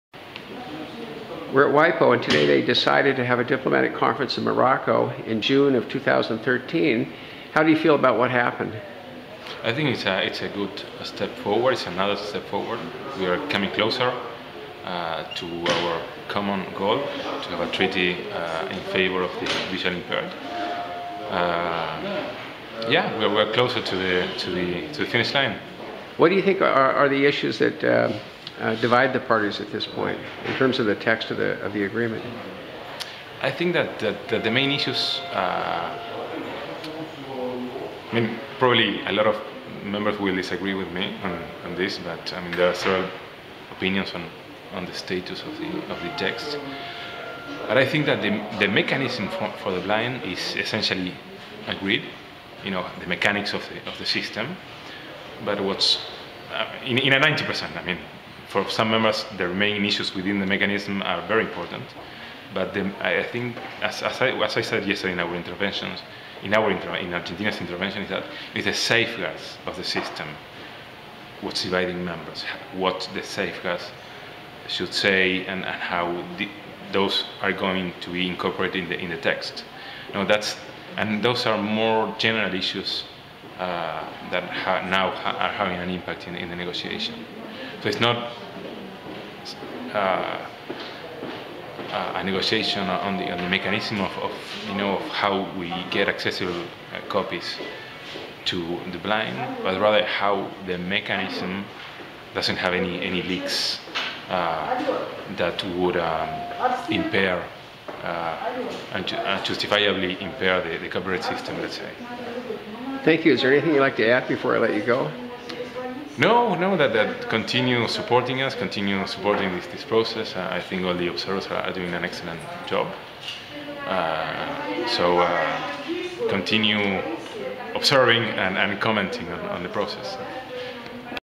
Rodrigo Bardoneschi on WIPO decision to convene diplomatic conference for blind treaty
Rodrigo Bardoneschi is a member of the Argentina delegation at WIPO.  He was interviewed on December 18, 2012, the day the World Intellectual Property Organization (WIPO) Extraordinary General Assembly decided "to negotiate and adopt a treaty on limitations and exceptions for visually impaired persons/persons with print disabilities" and to convene a diplomatic conference in June 2013 in Morocco.